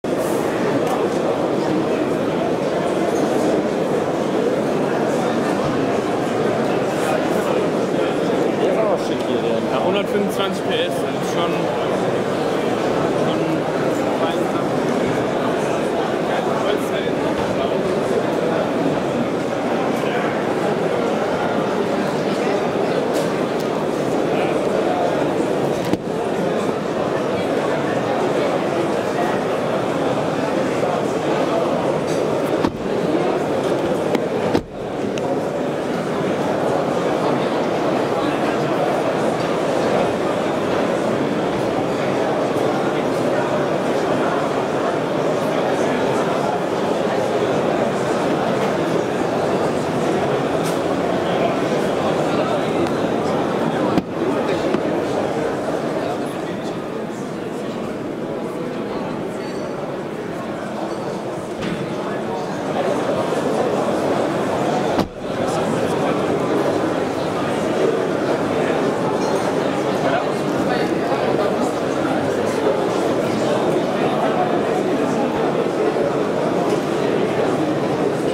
BRABUS Ultimate E Concept Exterior & Interior 204 Hp 180 Km/h 111 mph * Playlist
BRABUS Ultimate E Concept Exterior & Interior 204 Hp 180 Km/h 111 mph * see also ''SMART BRABUS MODELS''-Tailor Made-Ultimate Style-Crossblade-White Buffalo